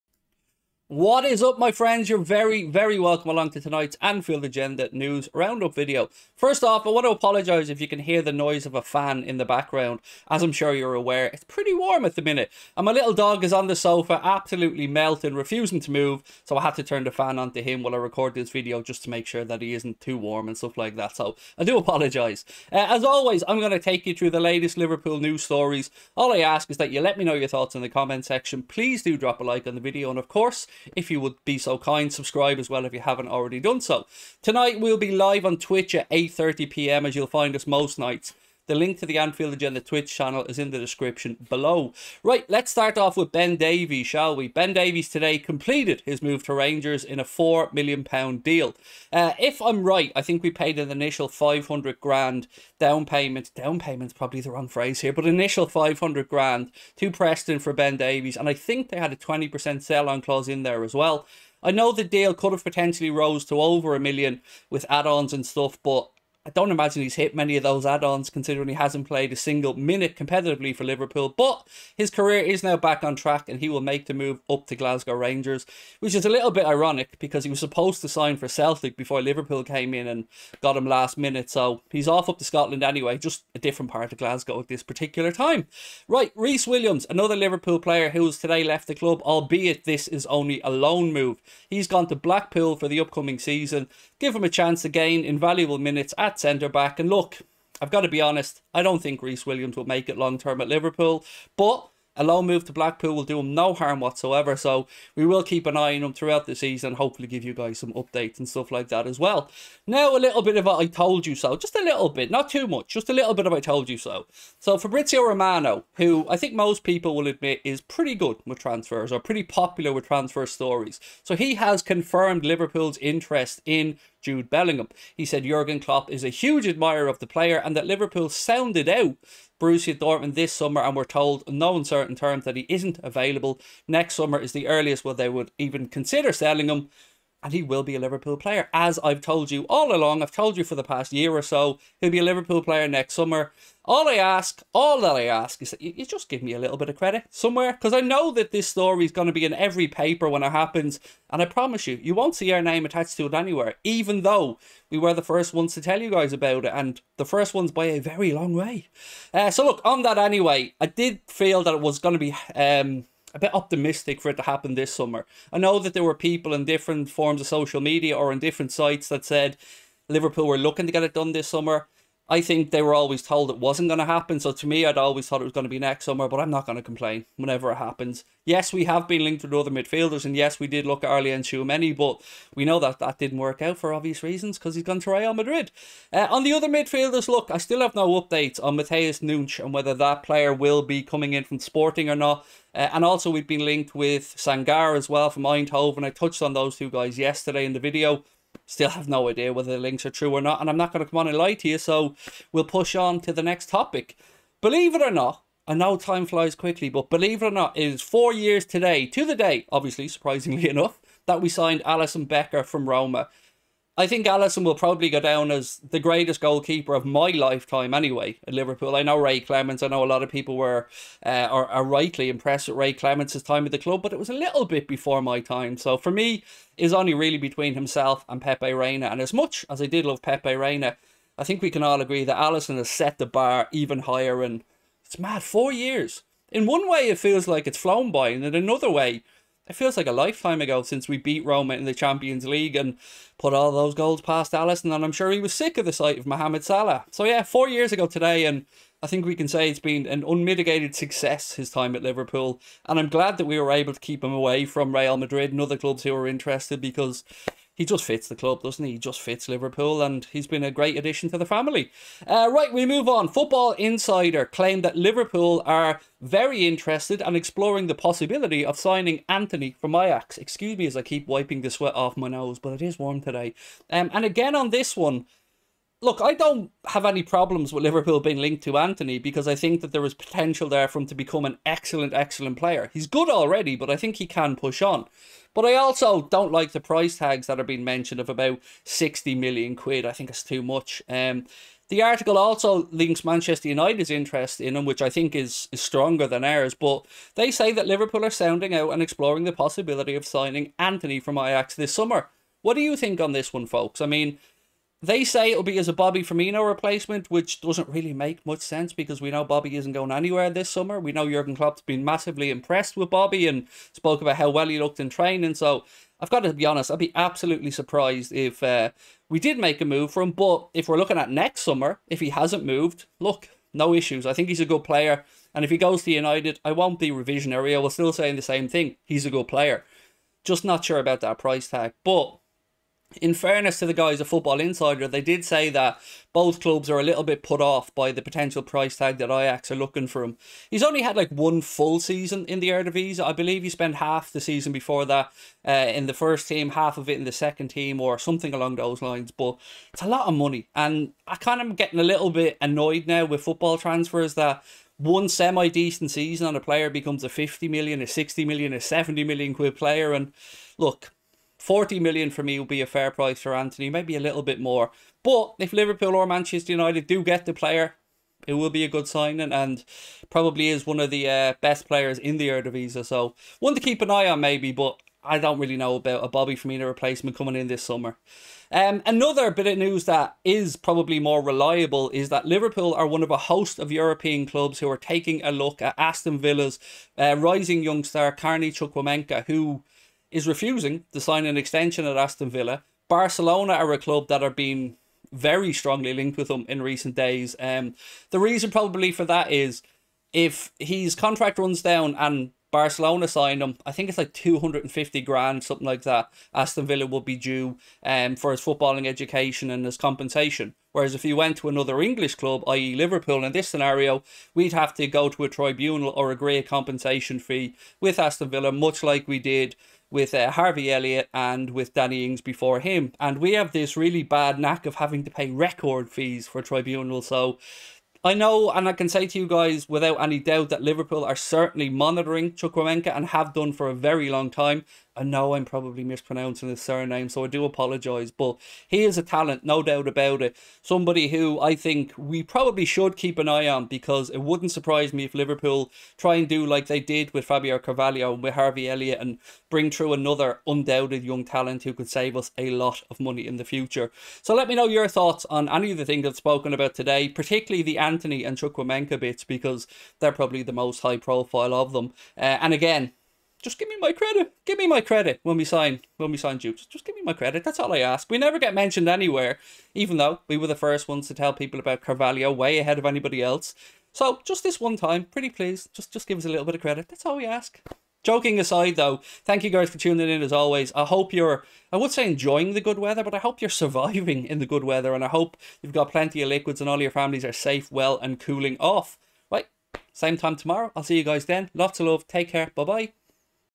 LIVERPOOL BEGIN TALKS WITH BELLINGHAM! Antony to Replace Firmino?
Liverpool have begun talks with Bellingham! Craig Houlden reacts to the latest Liverpool transfer news and rumours, including a very interesting update from Fabrizio Romano